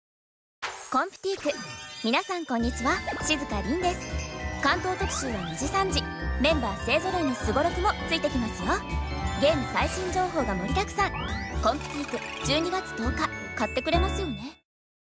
『コンプティーク 2019年1月号』 発売CM
「にじさんじ」2号連続表紙＆付録　第一弾！ 今月はJK組からのメリークリスマス！

表紙はねづみどし描きおろしの「にじさんじ」JK組！
巻頭特集の「にじさんじ」最新情報と、本誌調べによるファンからの意識調査をお送りします！ さらに付録には「にじさんじ　オールスターすごろく」も♪　もちろん、活動２周年を迎え、バーチャルYouTuberとして先頭を走り続ける「キズナアイ」特集など、今月もバーチャルYouTuber情報満載でお届けします♪ クリスマスシーズンを迎える「艦隊これくしょん -艦これ-」ゲーム情報や、２大コミック「水平線の、文月」「今宵もサルーテ！」にも注目です！
そして今月の「ぐだぐだエース（仮）」は、「FGO」ファン必見!!　加えて、本誌連載開始15周年を記念して、「らき☆すた」の美水かがみ描きおろしポスターも付録でついています。

【コンプティーク2018年1月号】
発売日：2018年12月10日

(C)KADOKAWA CORPORATION 2018 Printed in Japan